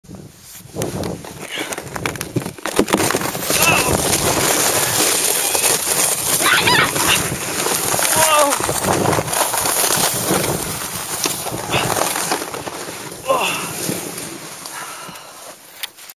Gary gives it a go
Gary goes down the ice slide